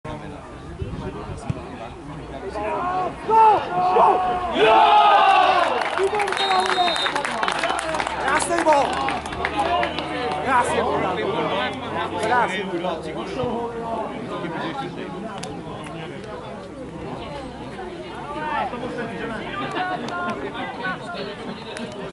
Chodov - Stod 7:0 - Gól Chodova na 4:0
20. kolo 1. B třídy sk. A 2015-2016
Chodov - Stod
Gól Chodova na 4:0